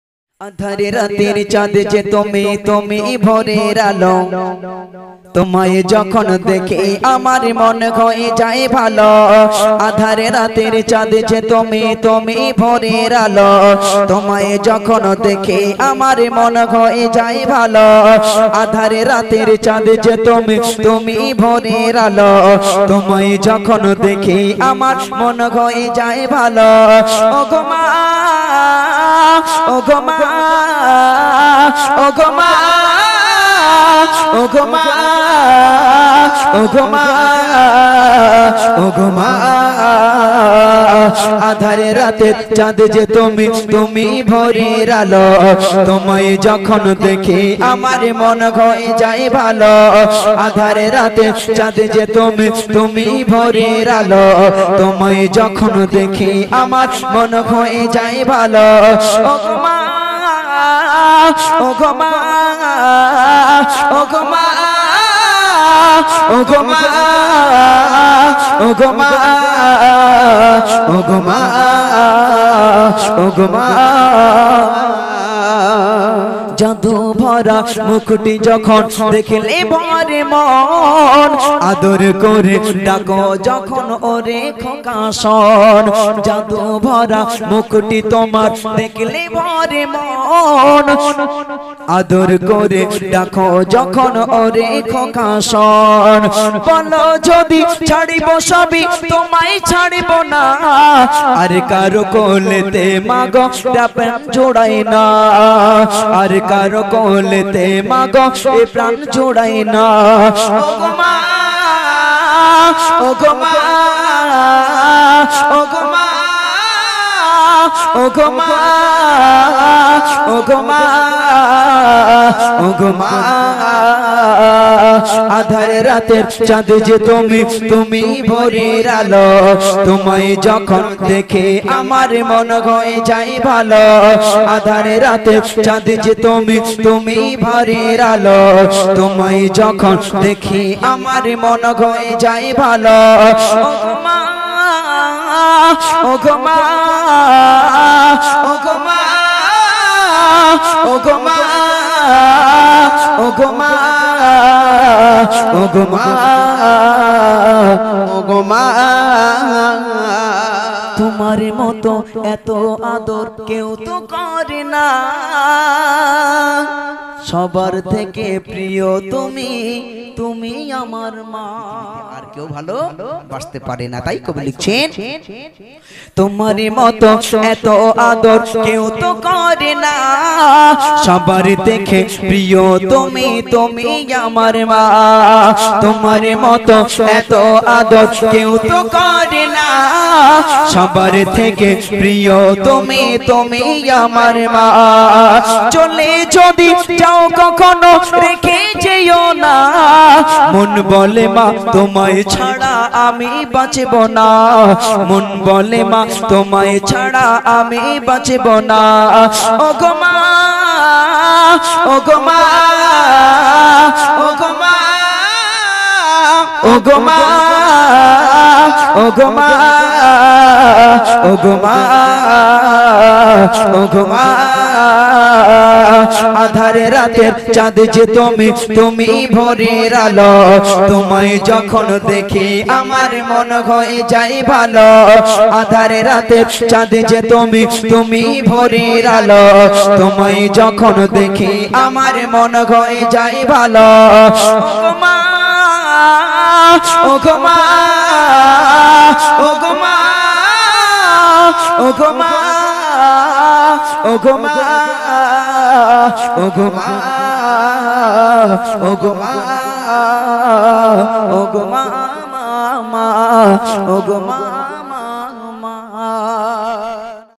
Islamic Naat. Gajol. সাইফুদ্দিন আমিনির কণ্ঠে একটি বিখ্যাত গজল।। MD Saifuddin Amini.
পৃথিবীতে মা ও সন্তানের সম্পর্ক যে এত মধূর। 'মা'যে আমাদের কত প্রিয় হয় এই গজলটি থেকে শুনুন।